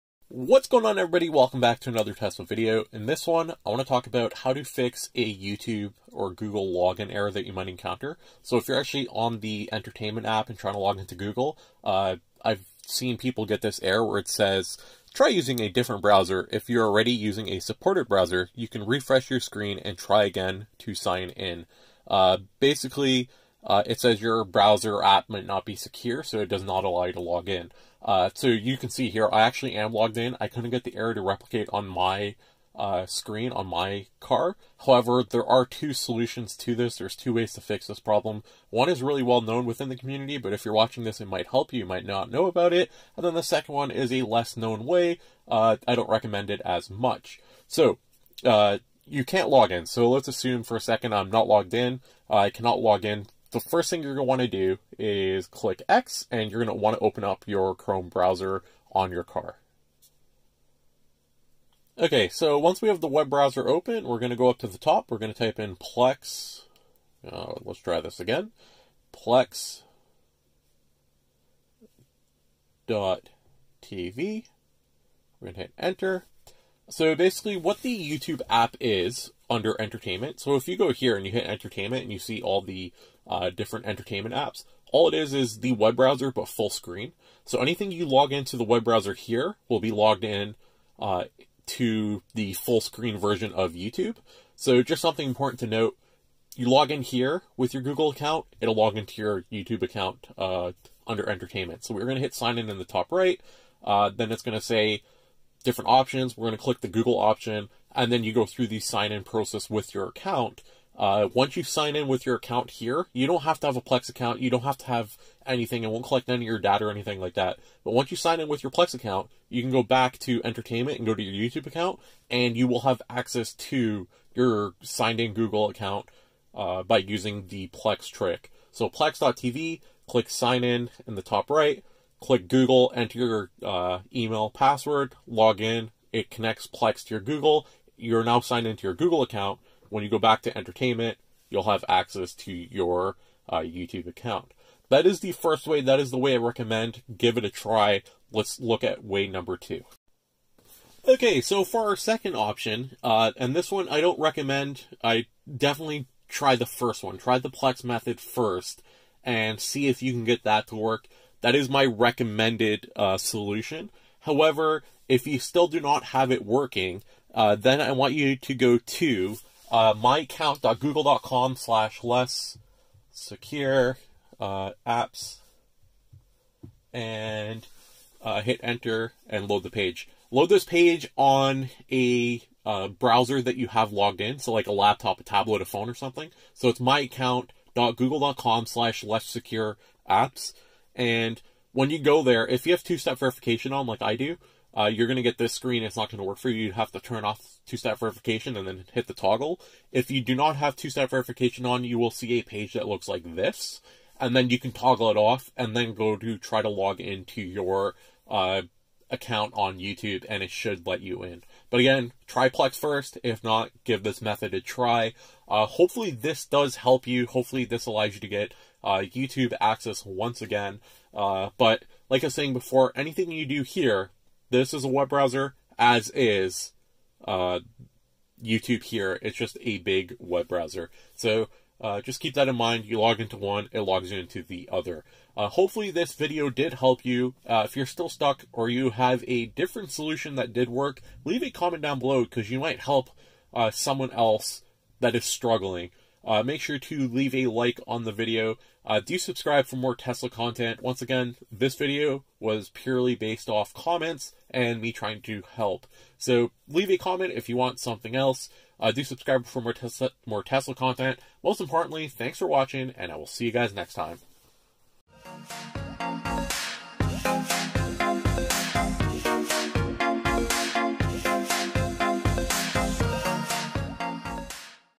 2 Ways To fix the Tesla YouTube Login Bug | Insecure Browser Error Message
There is a YouTube login bug in the Tesla Software. You will receive a message that says you are using an insecure browser and you will not be able to login. This is annoying, especially if you want to watch videos form your subscriptions (like my channel). There are two ways you can fix this problem;

1) The first method is based on using a third party application to login to your Google Account. The simplest way is to go to plex.tv in the browser on your Tesla. Click sign in the top right and then use your Google account. Since the YouTube app is just the web browser, your login settings are synced. This fix takes under 5 minutes. They key step is using “CAPTCHA” to prove to Google that you are not a bot.

2) The second method is not recommended. You must also not be using two step verification otherwise this will not work. on your phone, laptop etc where you are able to login to your Google account. Toggle that setting.

Side Note: this was a requested video from my comments. If there is something you want to see, please let me know. :)

Get 1,500 km of free Supercharging when you purchase a new Tesla! Copy the link below before placing your order: